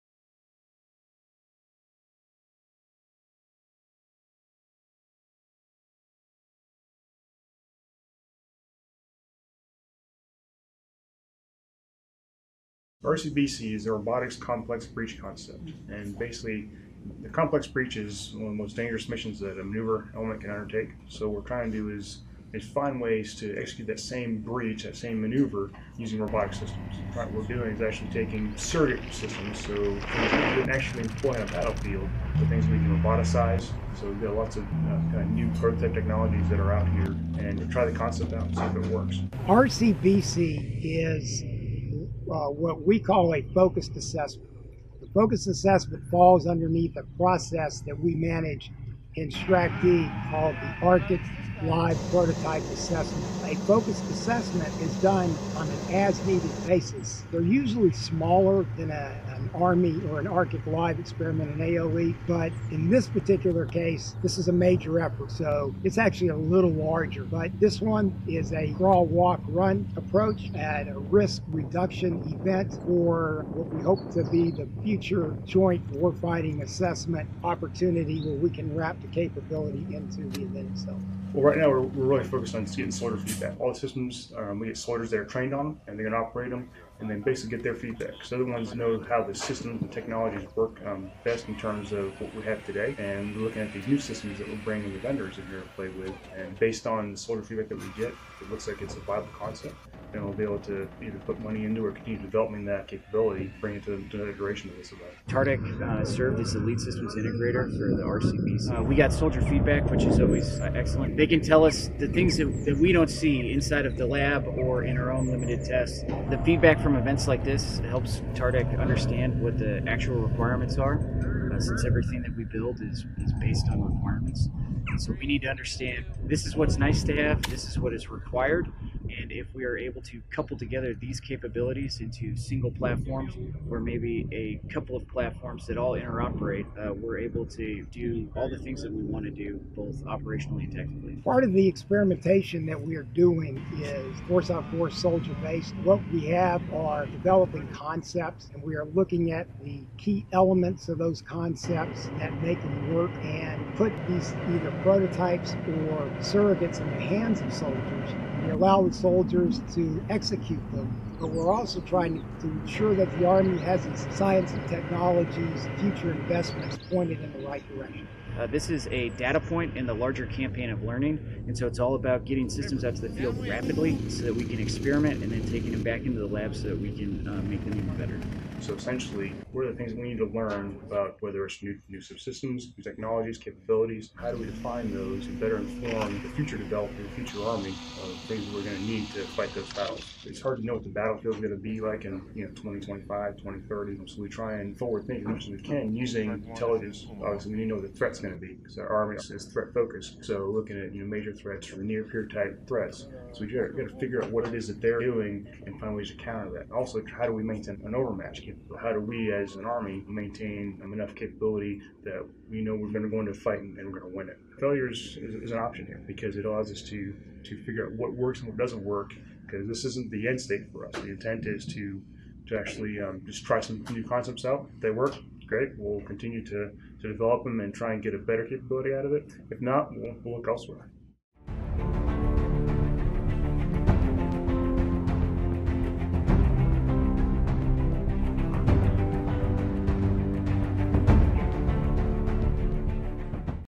RESEARCH and TRAINING to WIN FUTURE WARS! (Robotic Complex Breach Concept March 27-April 6, 2018.)
Research and training to WIN FUTURE BATTLES AND WARS! (Robotic and Autonomous Systems employed at Robotic Complex Breach Concept March 27-April 6, 2018.)

Official release: Robotic and Autonomous Systems (RAS) were employed during the Robotic Complex Breach Concept at Grafenwoehr, Germany, March 27-April 6, 2018, as part of Joint Warfighter Assessment 18.1. The U.S. Army Tank Automotive Research, Development and Engineering Center (TARDEC) functioned as the lead technology system integrator while conducting protocol experimentation at the risk mitigation event. TARDEC, along with other Department of Defense Organizations including Armaments Research, Development, and Engineering Center (ARDEC), Communications-Electronics Research, Development, and Engineering Center (CERDEC); Night Vision and Electronic Sensors Division (NVESD), PEO Missile Systems, Joint Project Manager for NBC Contamination Avoidance, Pacific Command (PACOM), and others provided systems for enhanced warfighting potential of Manned Unmanned Teaming for Ground (MUM-T (G)). Maneuver Battle Lab, with the Maneuver Center of Excellence, Maneuver Support Center of Excellence, Marine Corps Warfighting Lab, and TRADOC Analysis Center (TRAC) provided assessment and analysis. The team investigated RAS ability to enable formations in terms of reconnaissance, surveillance, and target acquisition (RSTA) capabilities, and assess the current state of supporting technologies. Results of this assessment will be utilized to advance continued technological developments and address the doctrinal, organizational, training, and materiel implications.

Credit: Staff Sgt. Kimberly Derryberry, United States Army.

Many thanks for visiting our channel,

UMC